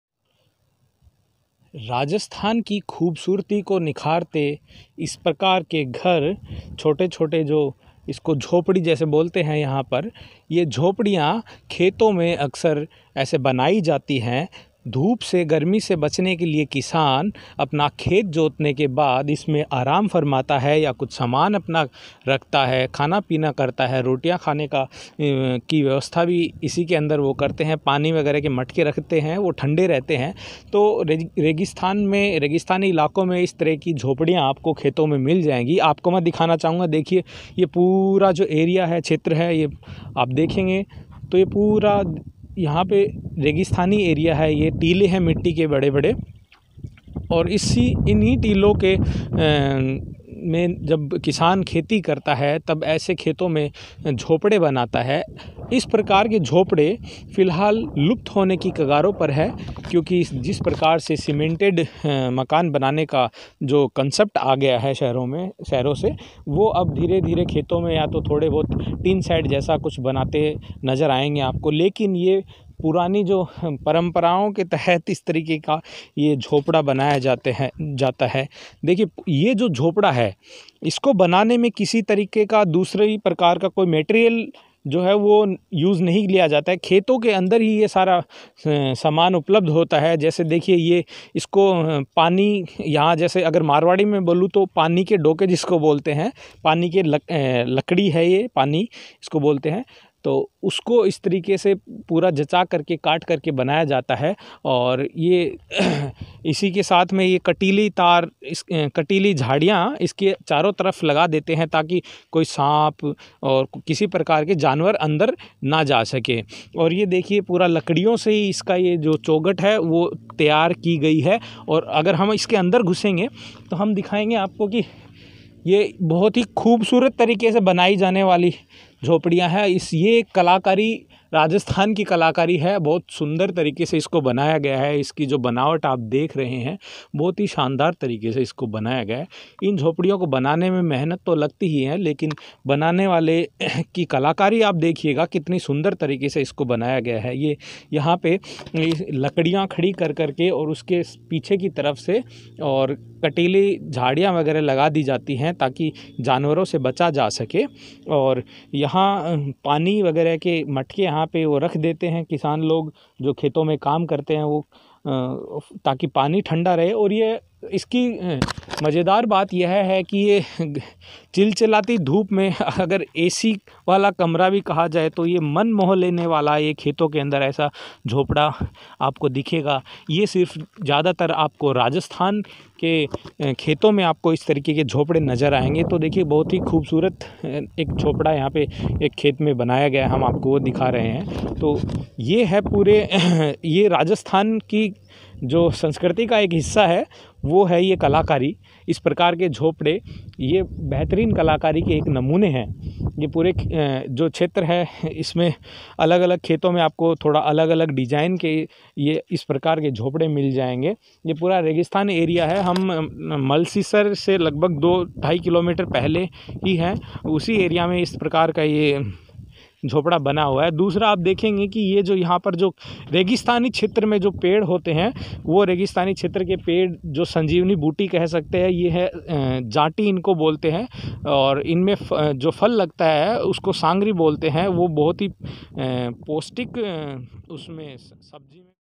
RAJASTHAN beautiful hut in rajasthan, AMAZING work..
राजस्थान की खूबसूरती को निखारता ये झोंपड़ा, इसकी खुबिया जान लो।